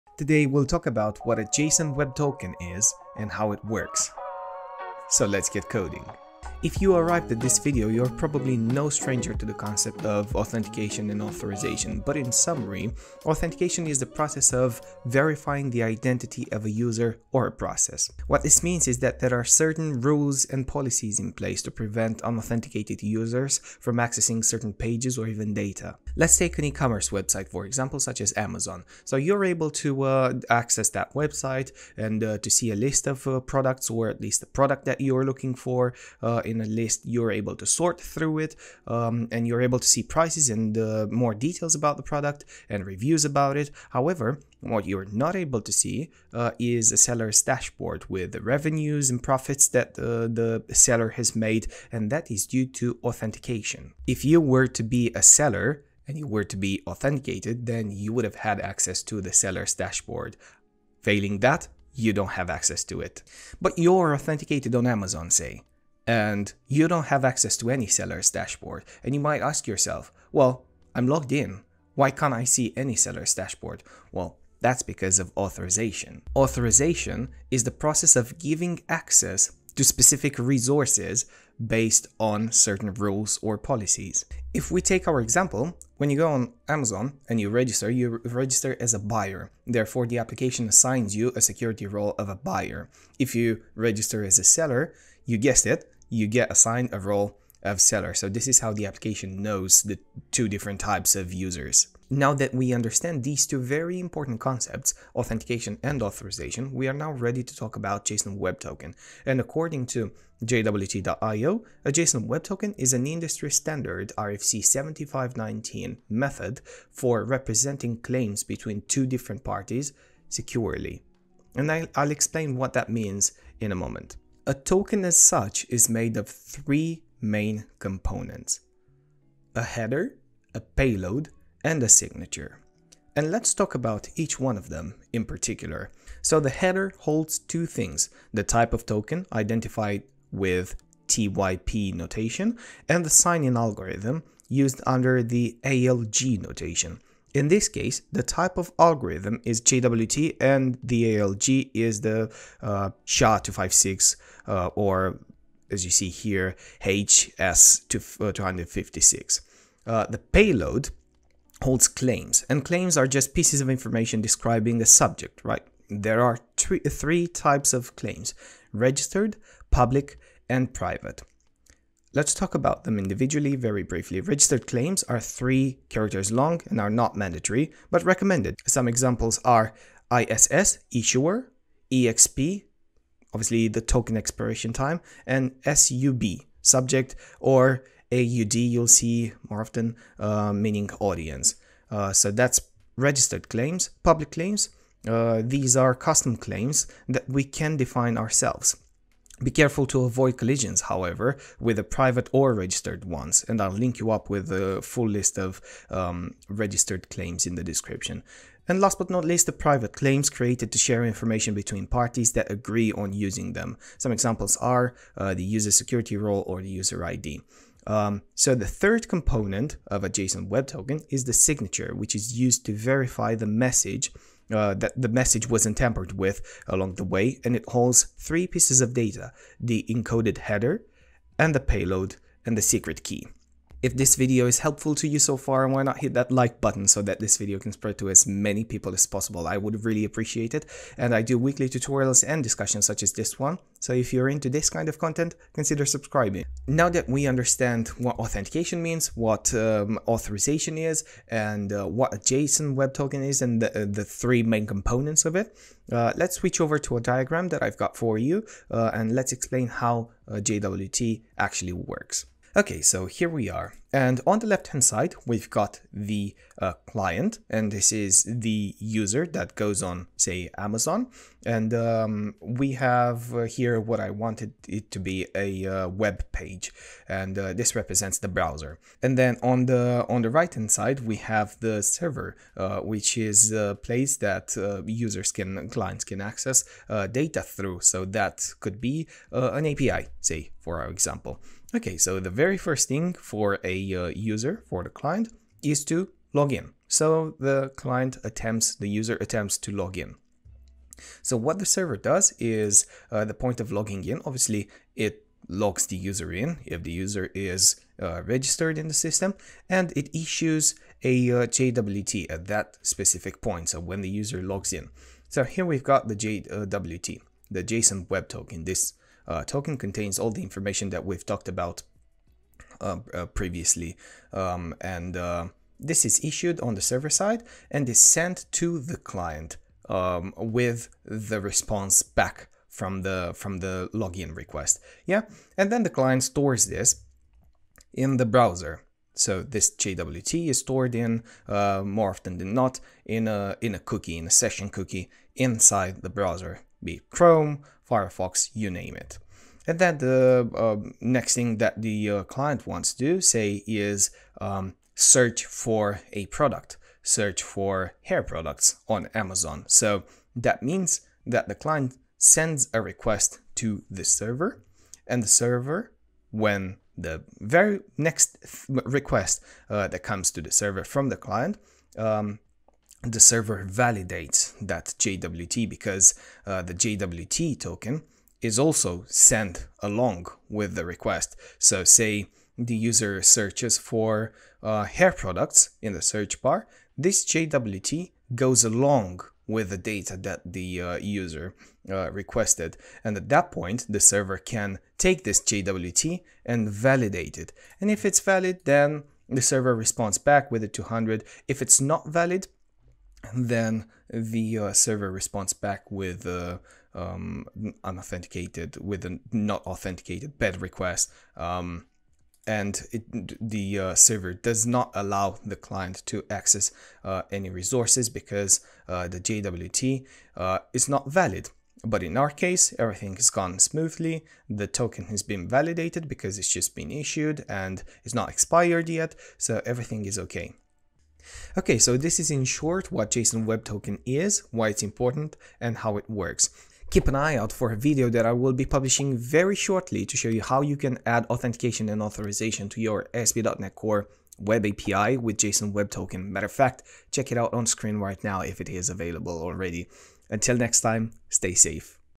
What is Json Web Token? JWT Token Explained
In this video we discuss what Json Web Token (JWT) is, why it’s important and how it works. We’ll start with an introduction on what Authentication is. Following, the authorization is explained and this is important because they are important concepts that JWT is built upon.

We’ll talk about how JWT Authentication and Authorization works. By the end of this video, you’ll have a good idea of what JWT is and how it works.